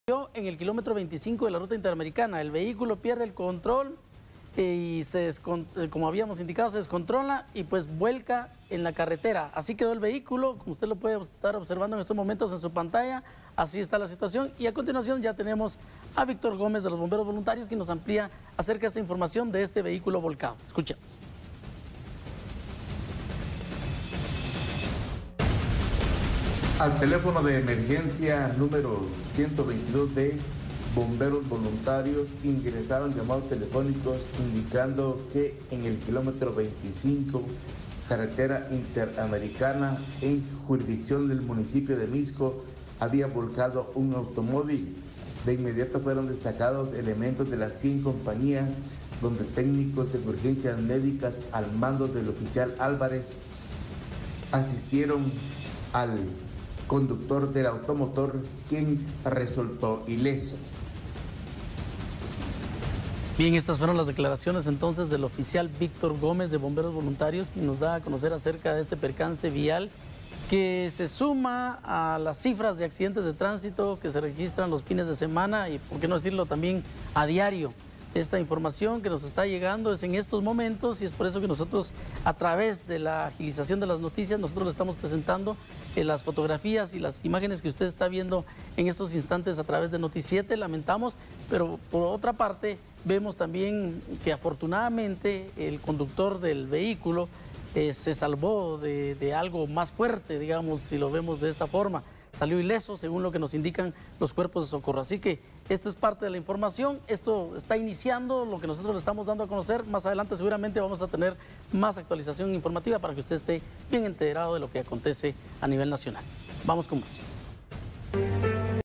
Conductor salva de morir tras volcar su vehículo en Ruta Interamericana
Automotor se despistó tras mala maniobra del piloto que resultó ileso pese al fuerte impacto que destrozó la parte frontal del automóvil.